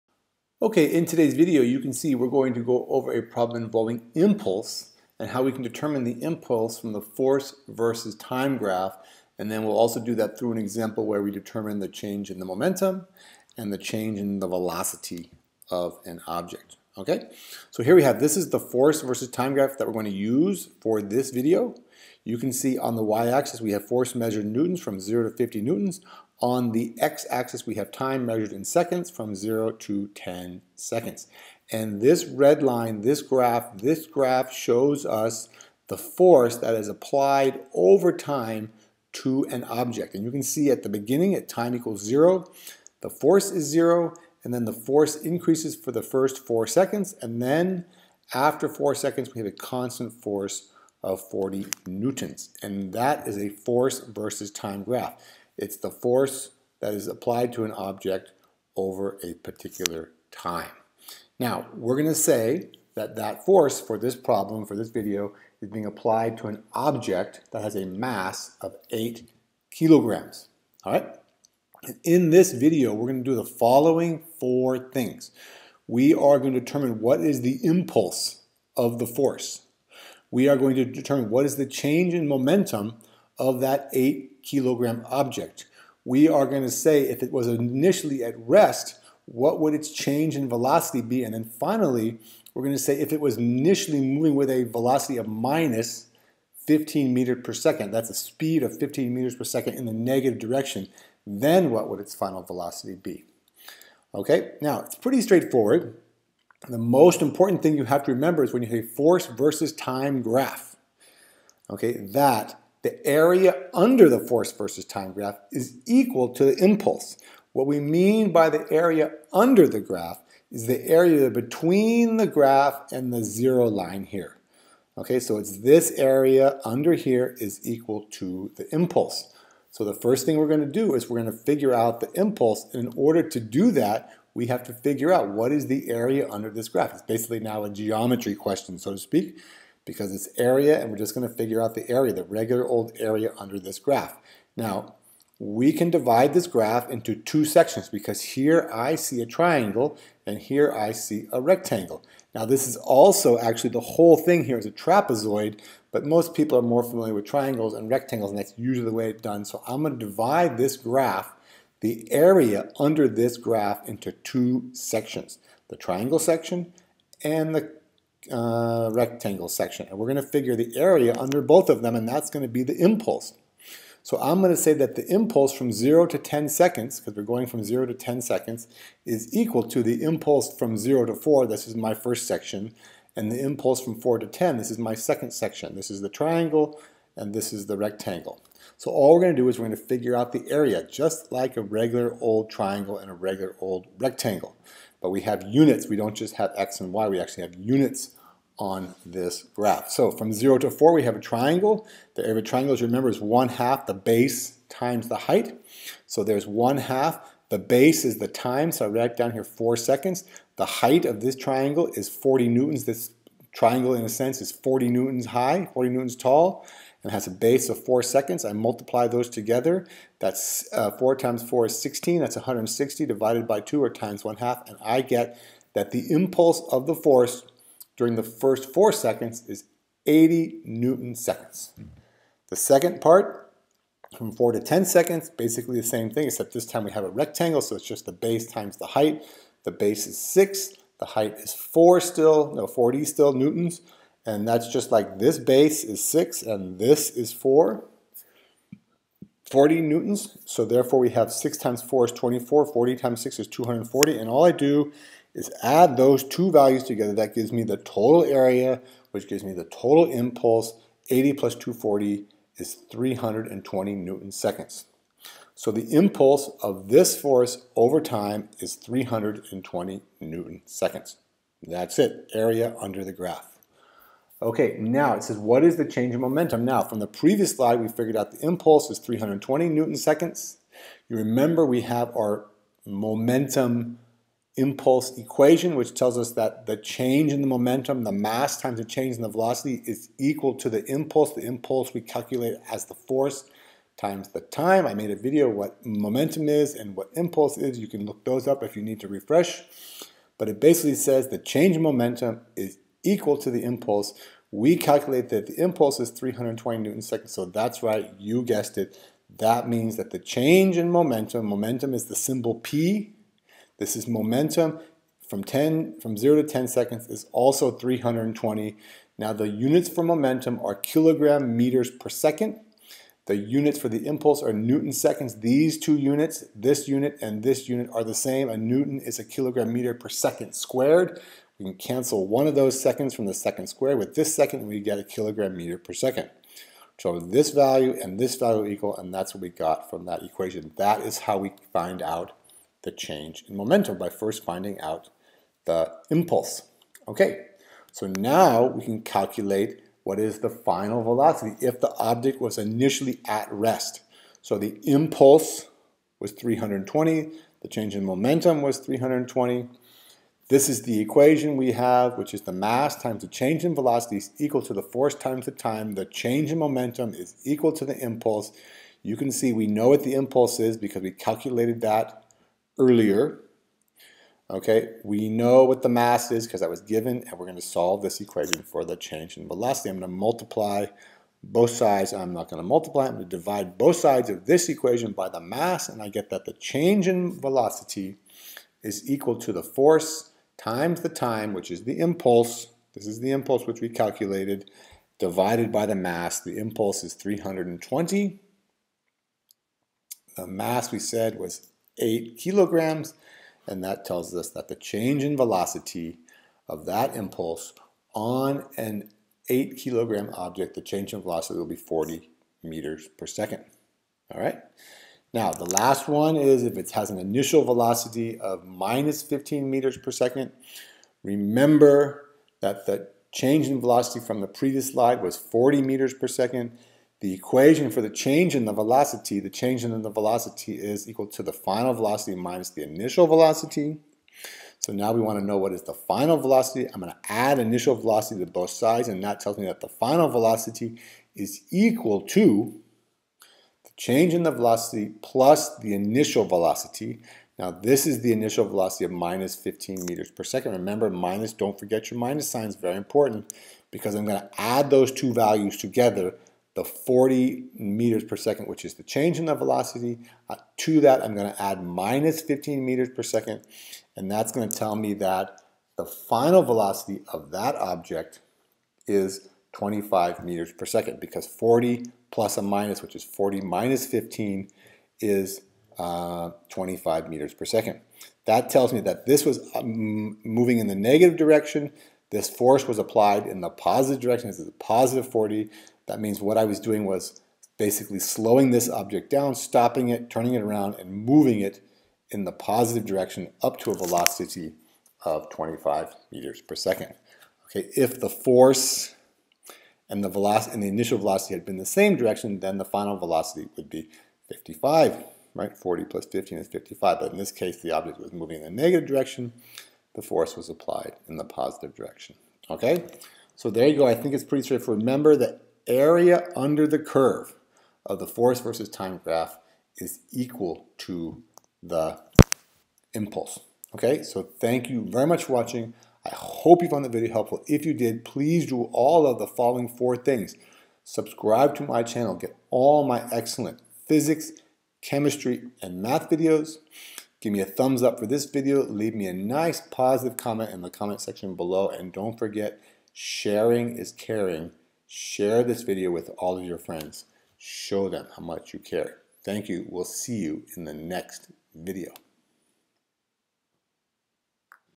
Momentum (4 of 16) Force vs Time Graph
This video goes over how to calculate impulse from the force vs time graph. The impulse is equal to the area under the force vs time graph. Also included is an example for calculating momentum and change in velocity.

Change in momentum is equal to the impulse. If you apply a force over a period of time then you will change the velocity of an object.

Momentum is a quantity of matter arising from its mass and velocity. The momentum of an object is directly proportional to its mass and velocity. Momentum is a vector quantity. In order to change the momentum of an object a force must be applied over time.

Impulse is the product force applied to an object and the time over which the force is applied. In the absence of friction, this will result in a change in the velocity of the object.

(3) Leave a nice positive comment